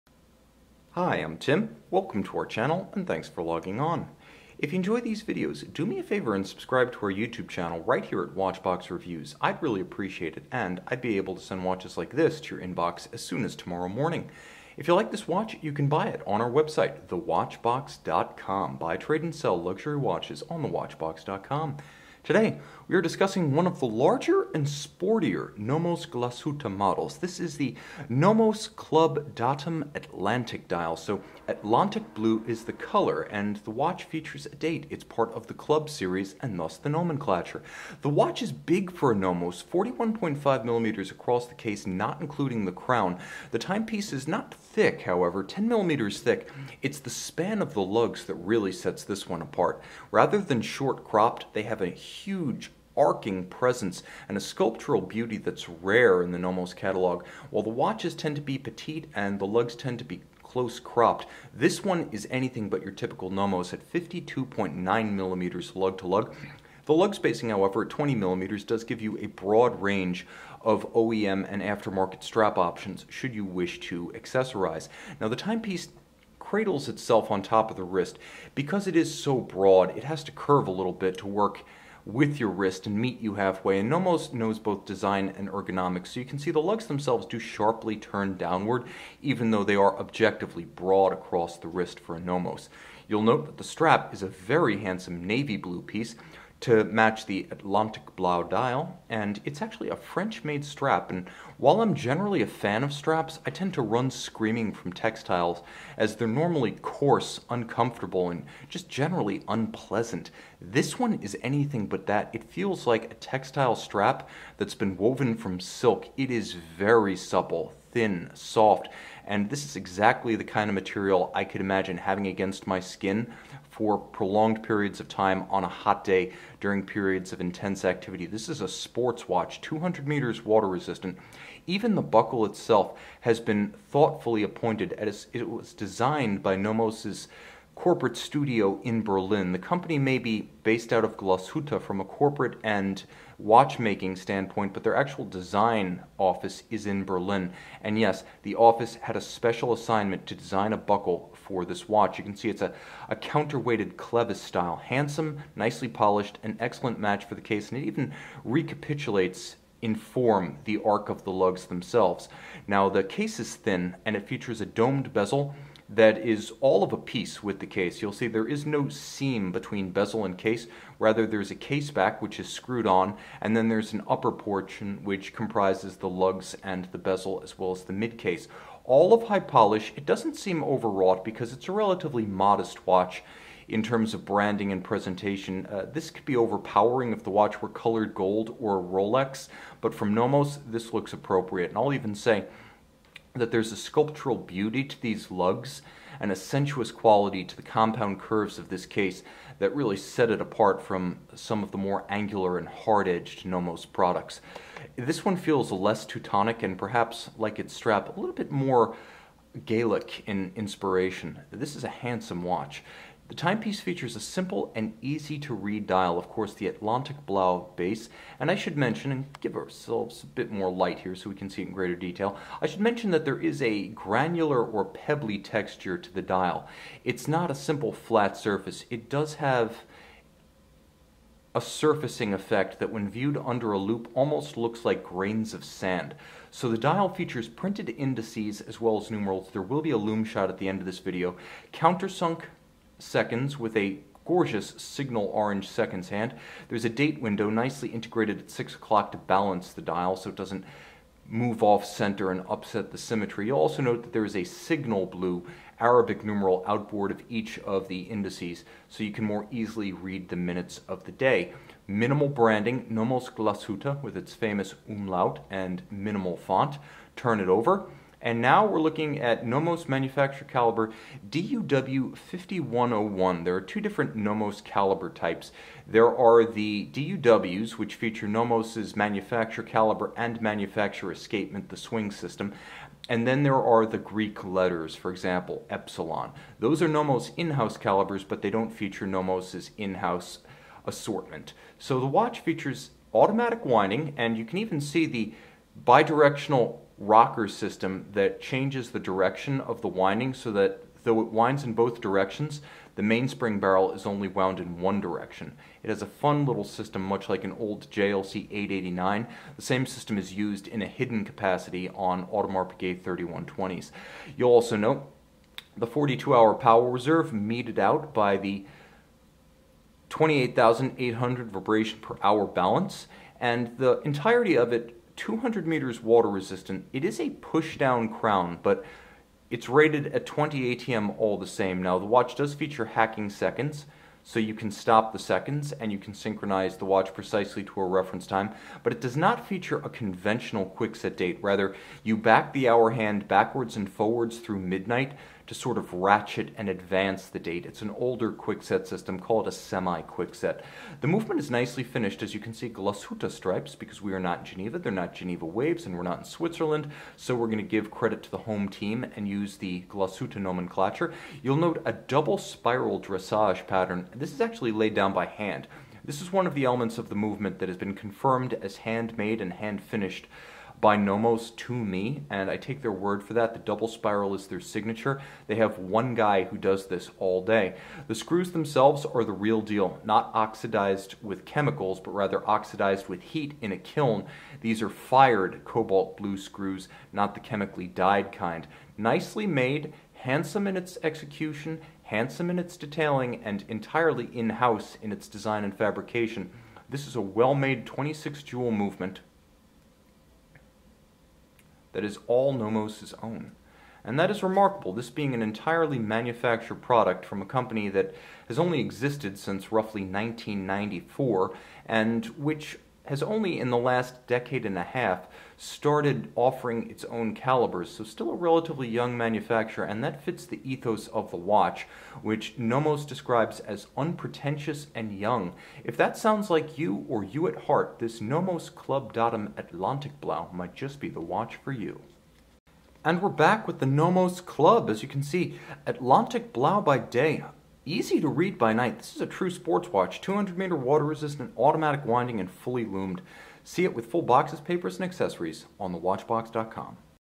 Nomos Glashutte Club Datum Atlantik 776 Luxury Watch Review
The Nomos Glashutte Club Datum Atlantik is encased in stainless steel surrounding a dark blue strap on a black textile strap. Functions include hours, minutes, and small-seconds. The watch measures 41.5mm in diameter, 10mm in thickness, and 52.9mm from lug-to-lug.